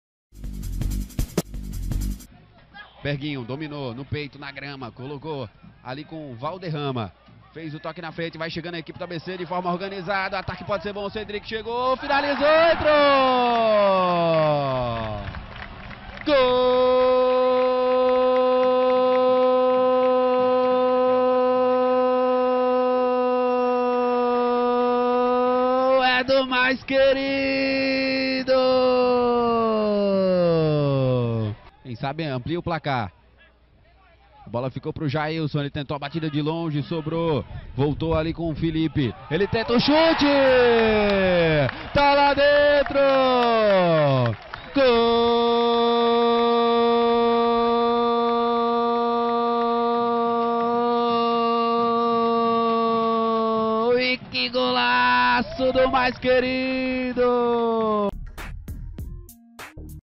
AMISTOSO | GOLS - ABC 2 X 1 BOTAFOGO/PB
⚫⚪ Confira os gols marcados por Cedric e Felipe Manoel, que garantiram a vitória contra o Botafogo/PB, em amistoso de pré-temporada realizado neste domingo (29), no campo do CT. Dois belos gols!

📹 TV ABC